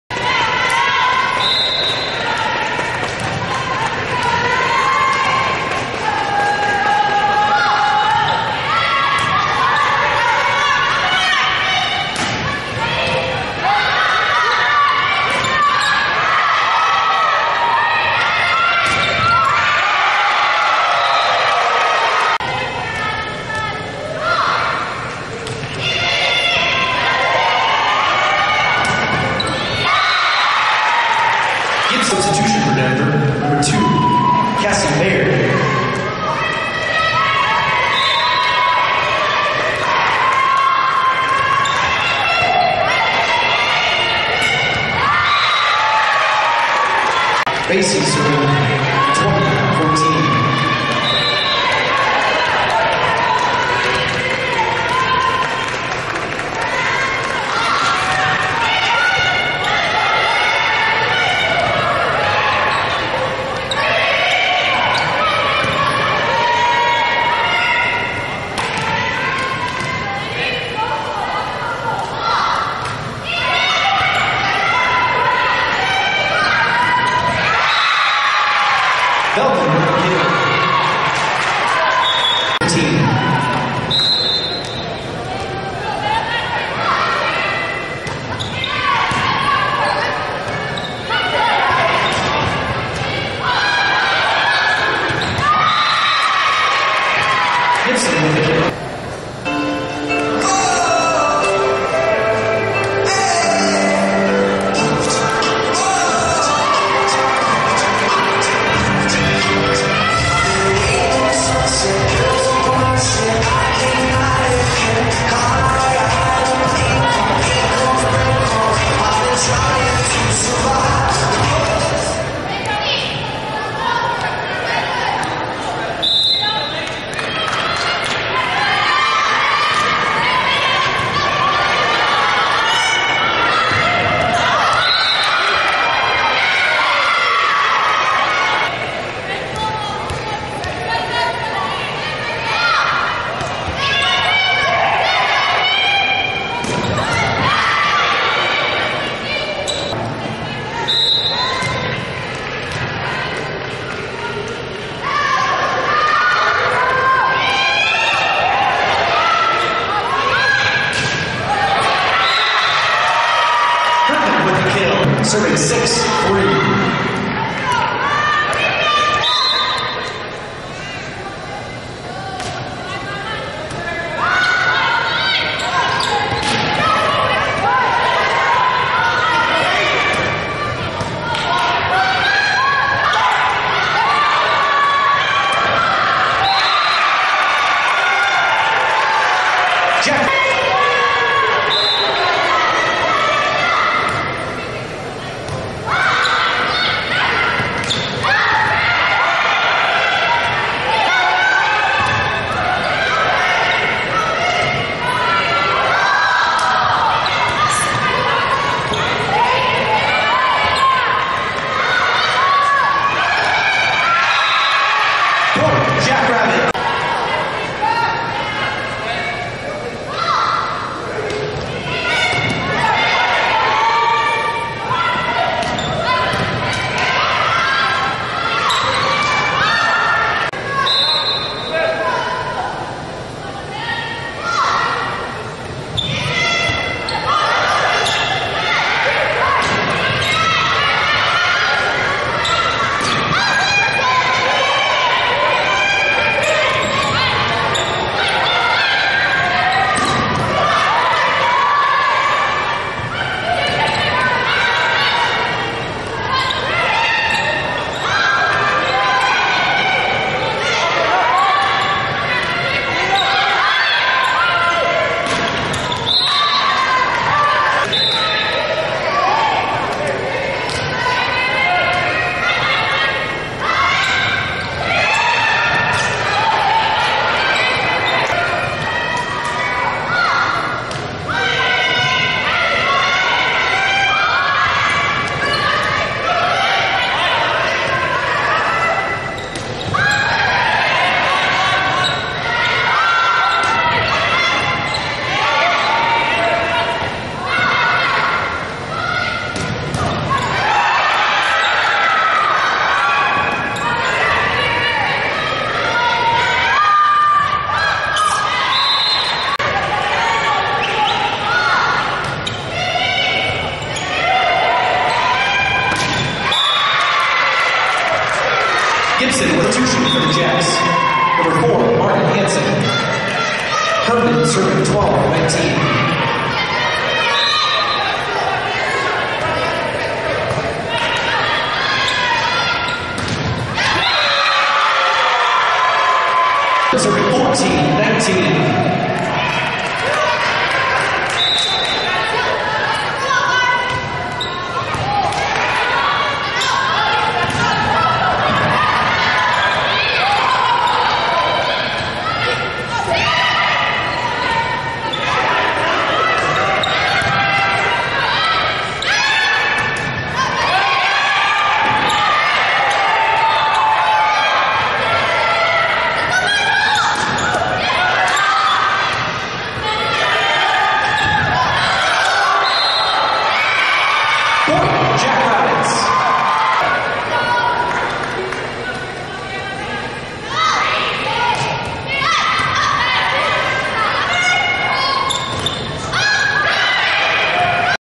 Tiara GIbson - Volleyball highlights vs Denver
Sophomore Middle Blocker #7
6'3" 10/3/14 - 7 kills...0.333 hitting...3 block assists